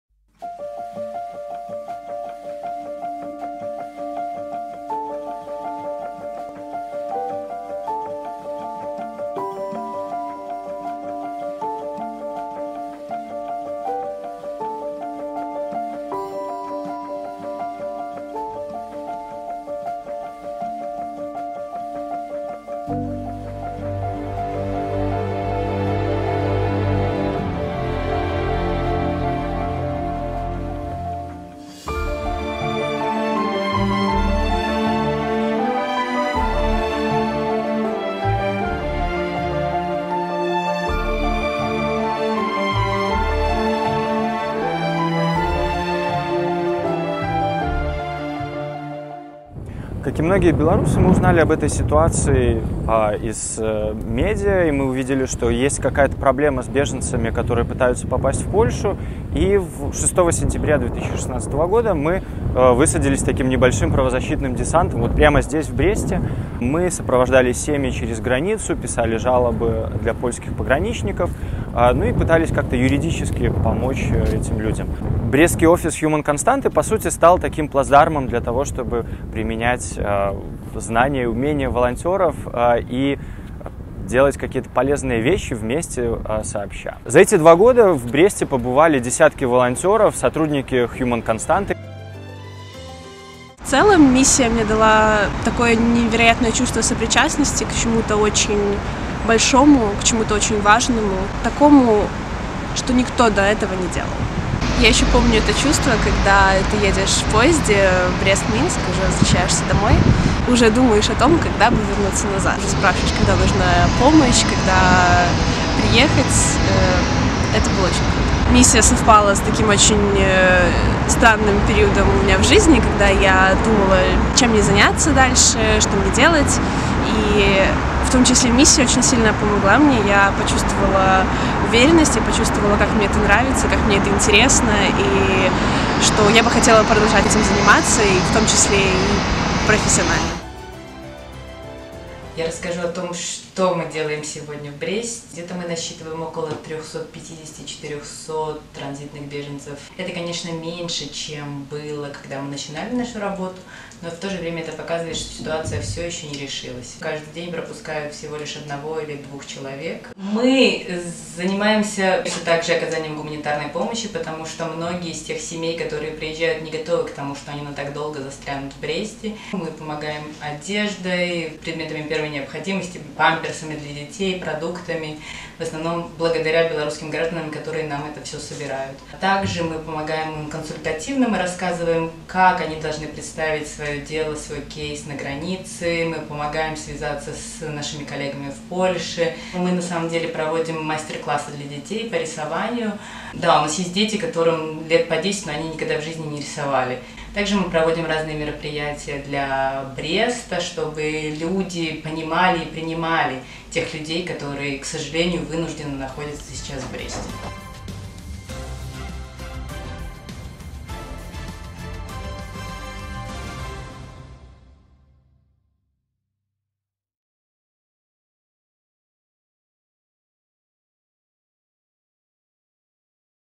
2 года миссии в Бресте | Human Constanta
6 сенября 2016 года команда Human Constanta высадилась в Бресте. Мы знали, что в тот момент на границе застряли около 3000 человек, которых не пускают в Польшу для получения убежища. Нам казалось, что проблема в каком-то простом недопонимании ситуации — со стороны беженцев или польских пограничников. Мы даже думали, что сможем решить проблему за какую-то пару недель. Миссия работает до сих пор.

Как мы начинали, что делали наши волонтёры и как мы работаем теперь?